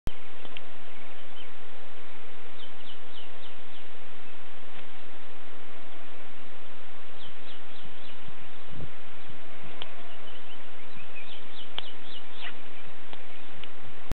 Scenery in Turkey
Beautiful scenery at the spot where we found Lyciasalamandra luschani.